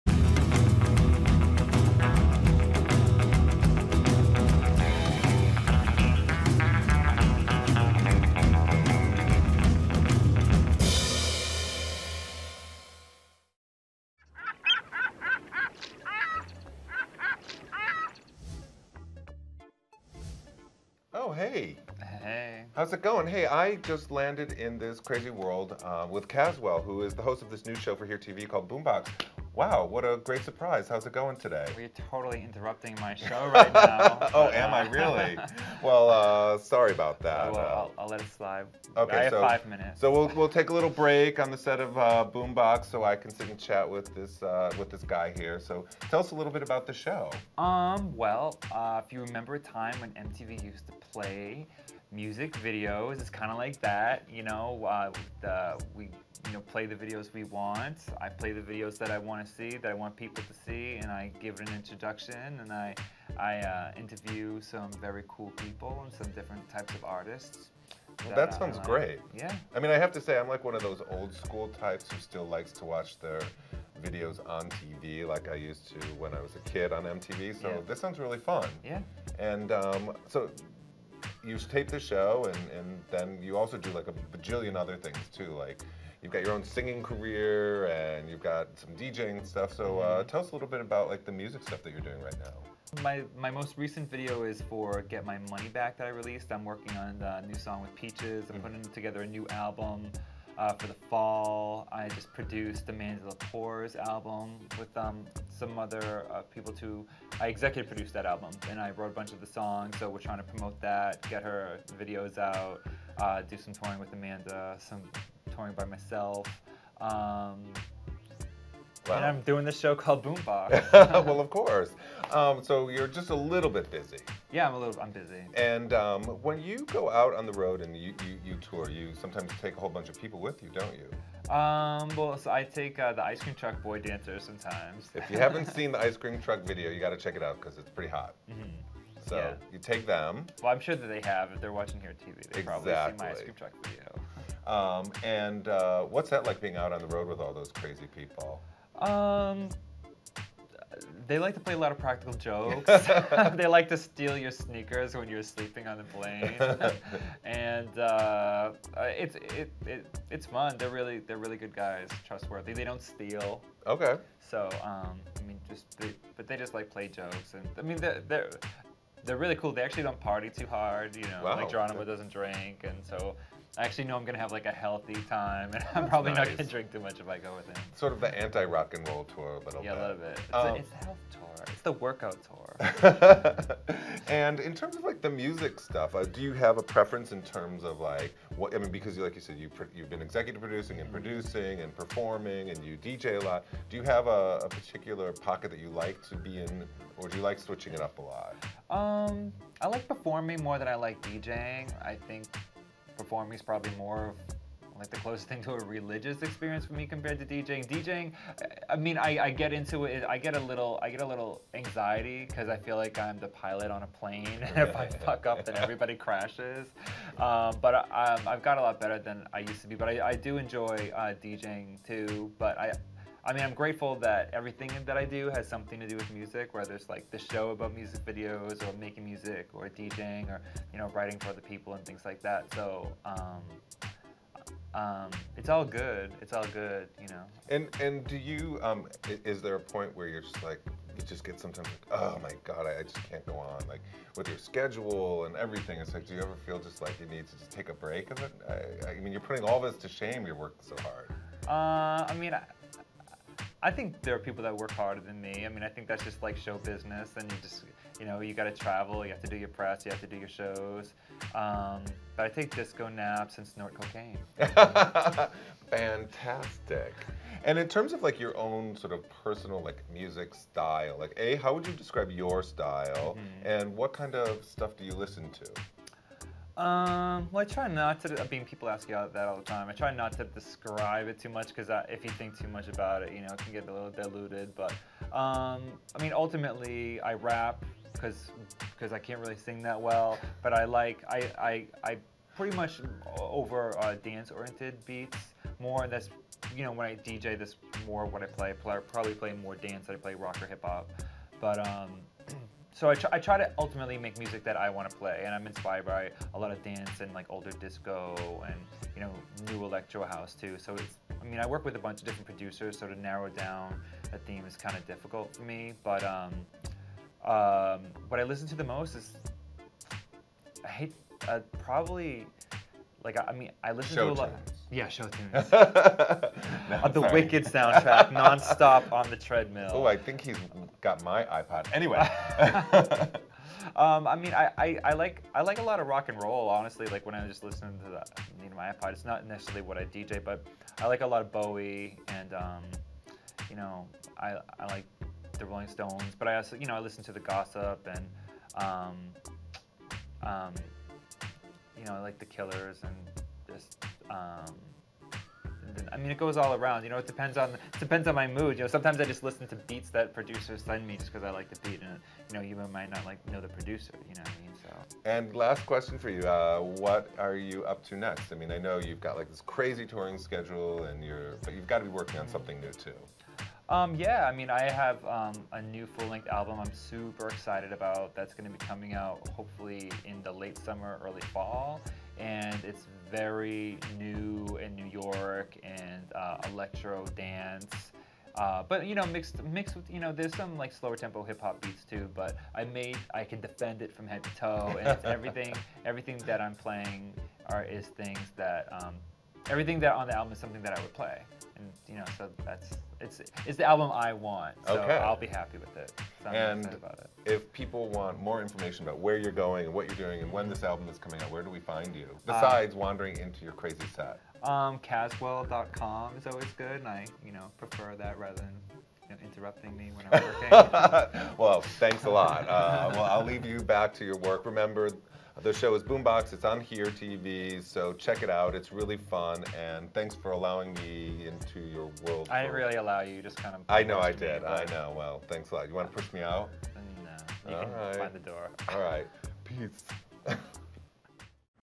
Just Josh 112: Cazwell
Josh drops in on Cazwell to discuss his new show Boombox on here! TV! Check it out!